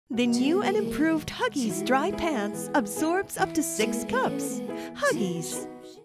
Huggies Dry Pants - The Philippines' Driest Diaper Pants
The NEW & IMPROVED Huggies Dry Pants can absorb up to 6 cups*, to keep baby dry and comfy!

* Based on average wetting of baby in 12 hours per XL size.

ASC Ref. No. K085P091718H